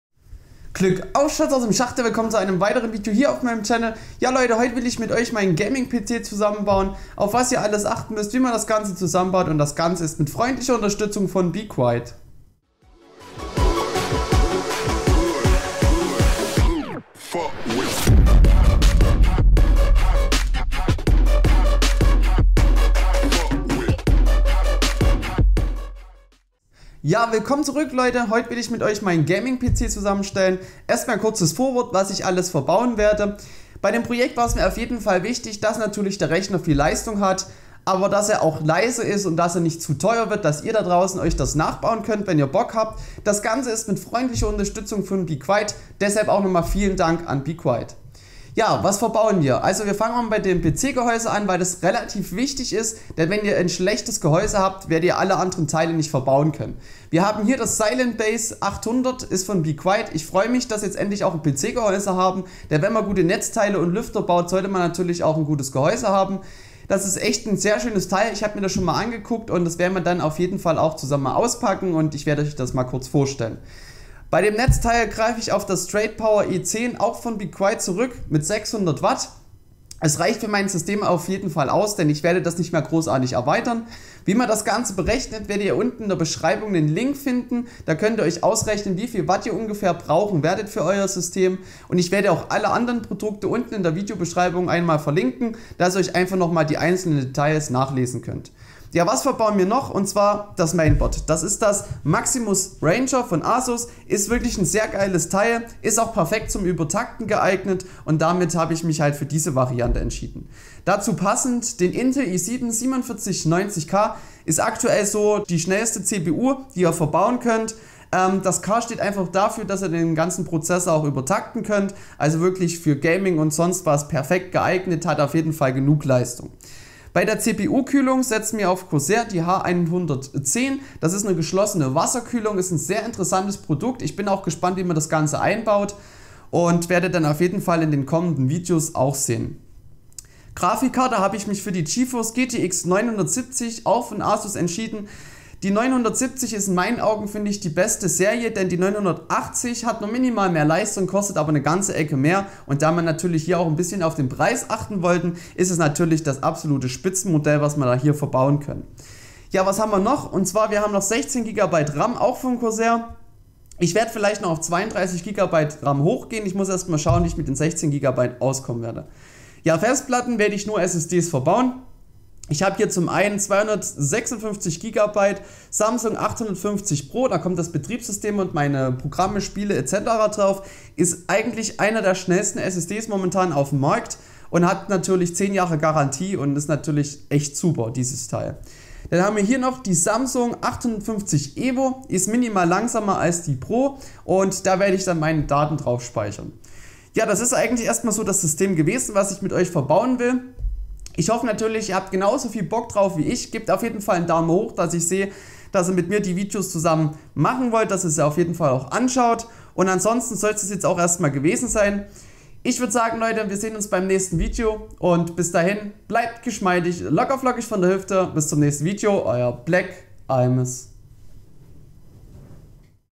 HIGH END GAMING PC SELBER BAUEN #1 | Tutorial - Asus, Corsair, be quiet!, Samsung
High End Gaming PC für kleines Geld selber bauen #1 | Tutorial - be quiet!, Asus, Corsair, Samsung | powerd by be quiet!
Vielen Dank an be quiet!, die mich bei diesem Projekt super unterstützt haben!!! Alle Produkte und Links findet ihr unten in der Beschreibung ;)

✘ Gehäuse ✘be quiet! Silent Base 800
✘Grafik ✘Asus Geforce GTX 970
✘CPU ✘Intel i7 4790K
✘Mainboard ✘Asus Maximus Ranger
✘Netzteil ✘Straight Power 10
✘SSD ✘Samsung 850 PRO 250GB
✘SSD ✘Samsung 850 EVO 500GB
✘RAM ✘Corsair Vengeance PRO 16GB
✘CPU Kühlung ✘Corsair H110